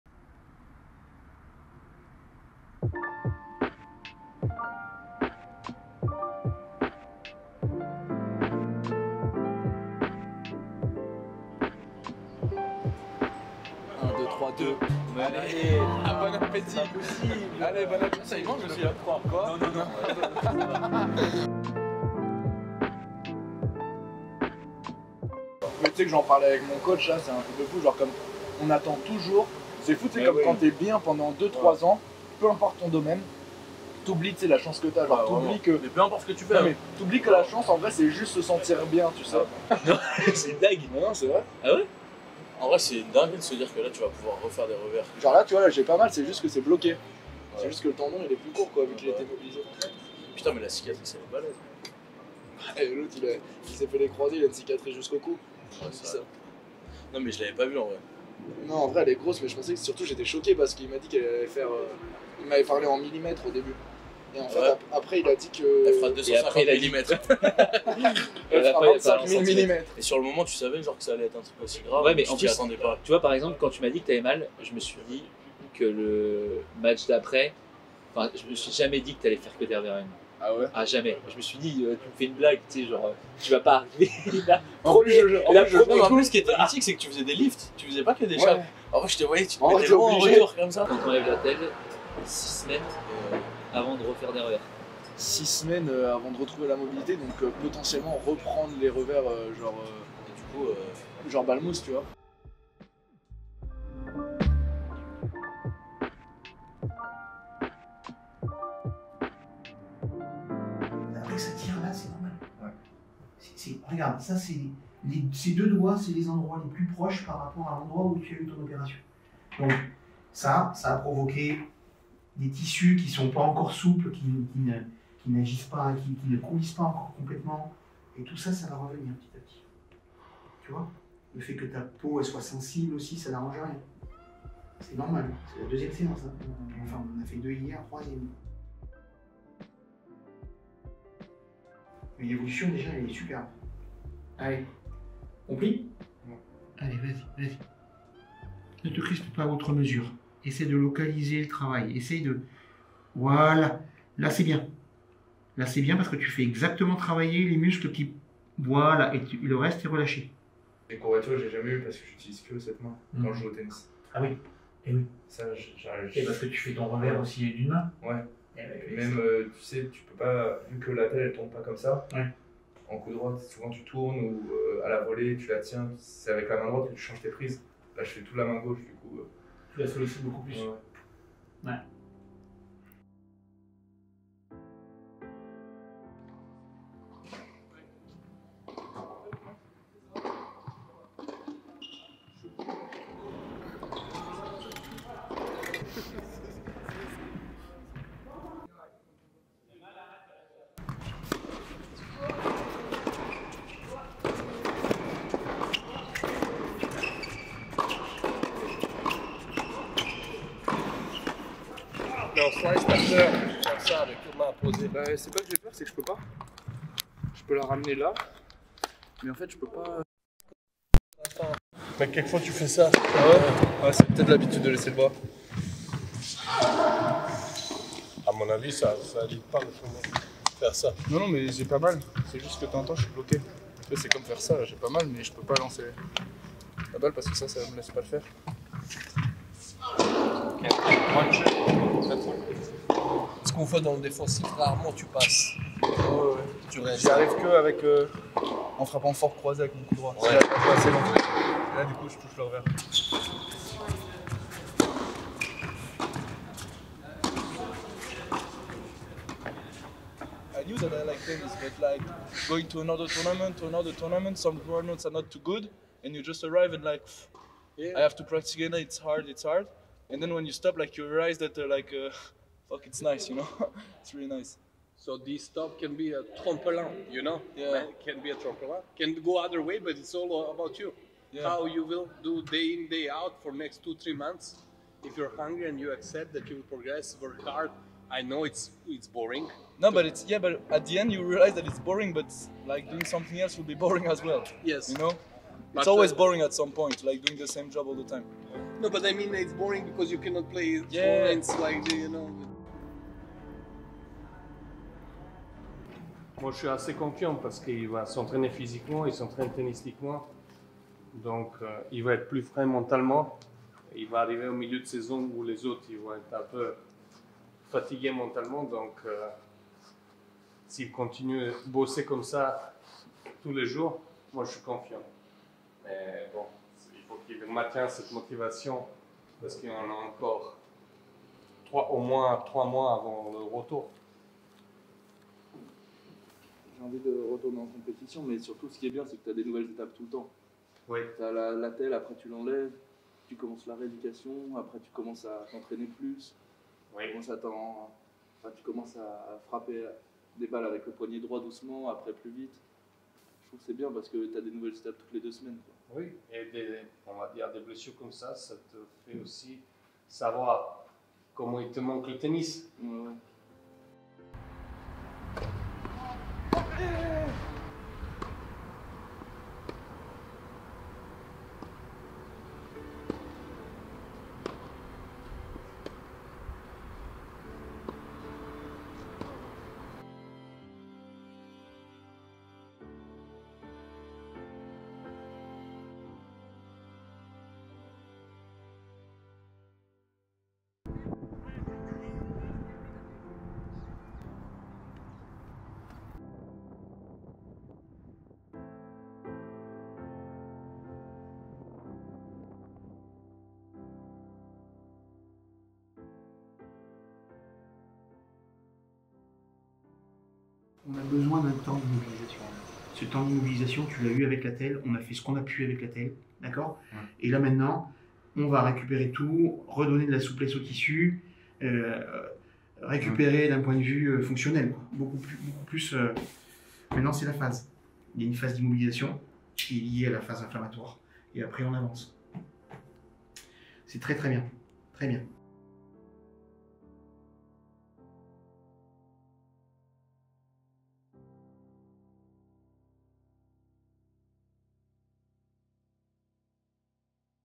ICEBERG - LA CONVALESCENCE
Bienvenue dans la partie submergée de l'ICEBERG🧊

À ma blessure j'ai décidé que ce serait le moment de vous montrer le quotidien d'un athlète quand il se blesse.
Une partie rarement montré par les caméras et pourtant souvent présente dans la vie d'un sportif

On glorifie souvent un athlète par ses résultats et les trophées qu'il soulève mais derrière les titres il y a le chemin.
C'est ce chemin qui m'intéresse et qui me tient à coeur de vous faire découvrir à travers ces quelques épisodes

j'espère que vous prendrez autant de plaisir à les regarder que j'en ai pris à les tourner 🙏
Bon visionnage,
À très vite sur les courts 🫡

Merci à Palomino Production pour avoir réalisé et monter l'entièreté de l'ICEBERG ! 🎥